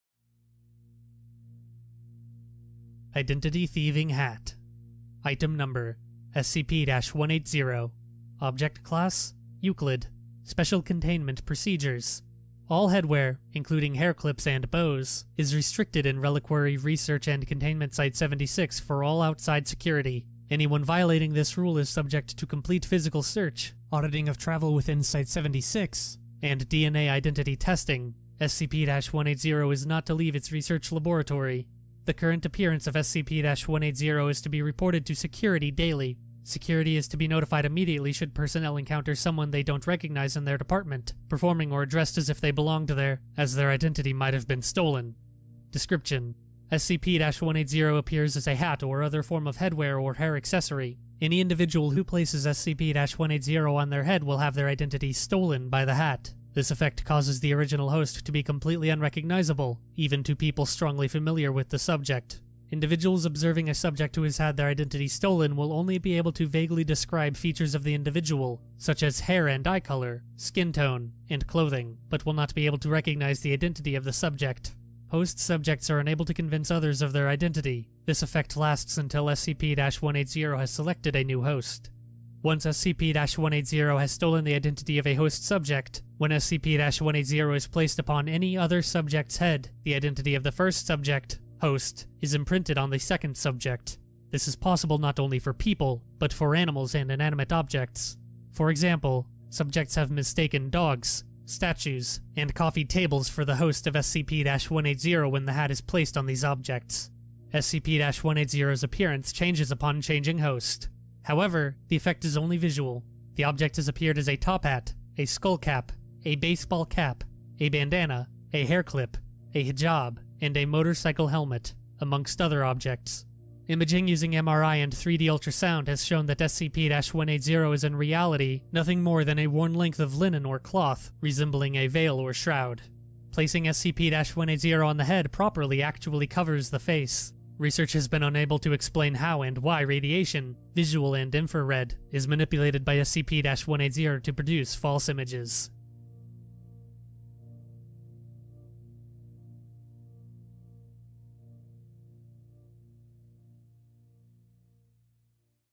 SCP-180 "Identity Thieving Hat" [SCP Wikidot]
I've heard of wearing many hats, but this is ridiculous!

Article by "FritzWillie".